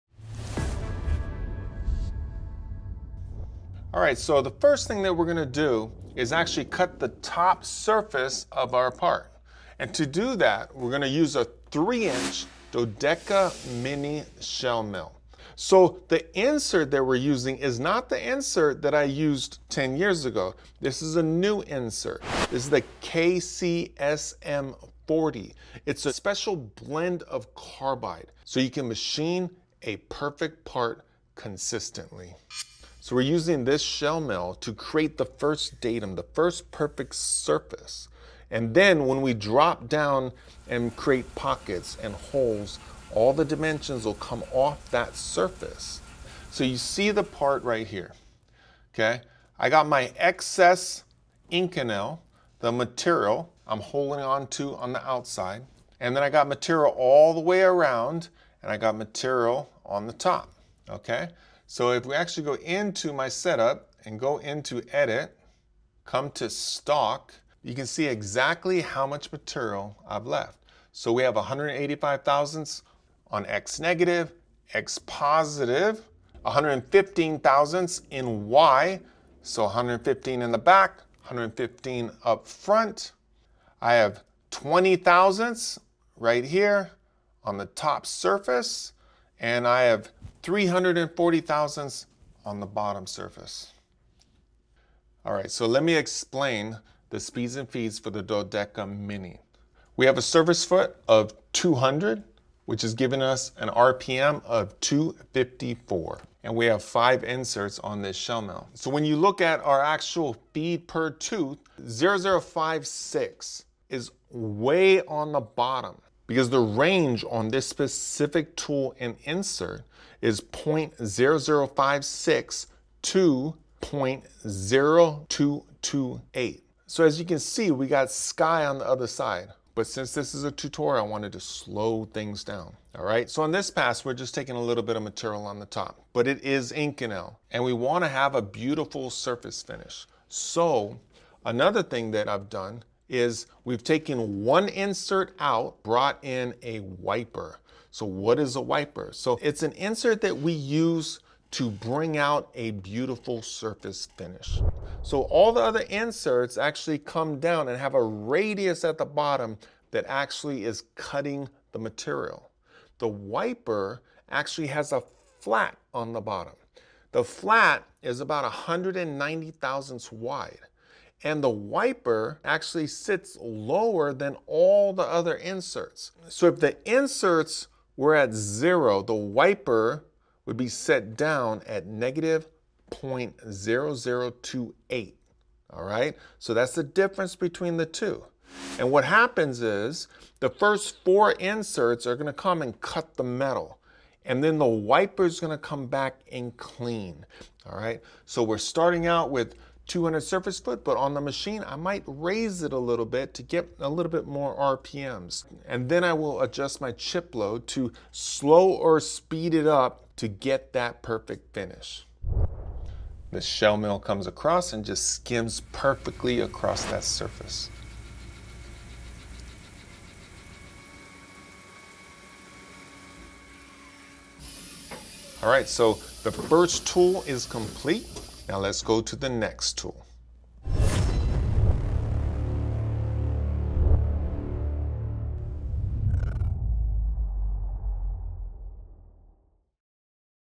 Face Milling Inconel 625 - CNC Machining - Kennametal Dodeka
CNC Machining Inconel 625 - Titan teaches you how to Face Mill Inconel 625. During this Advanced CNC Machining lesson Titan will share a few secrets concerning surface finish and explains how to have success in a low powered CNC.